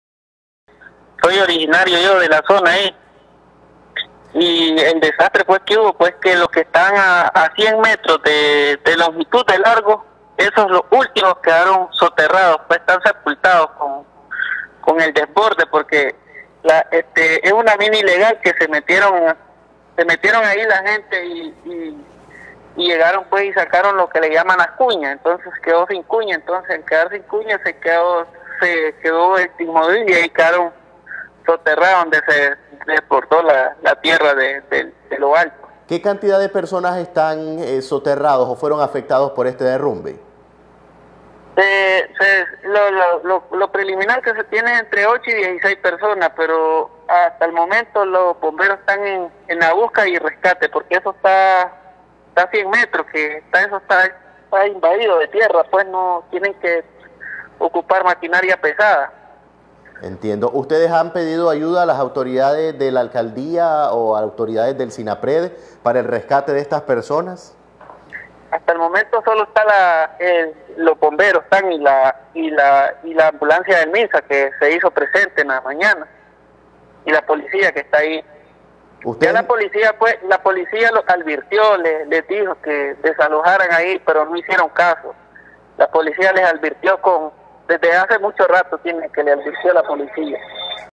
Derrumbe en mina de Rancho Grande deja a varias personas soterradas
NosResistimosAlEmbargo Derrumbe en mina de Rancho Grande deja a varias personas soterradas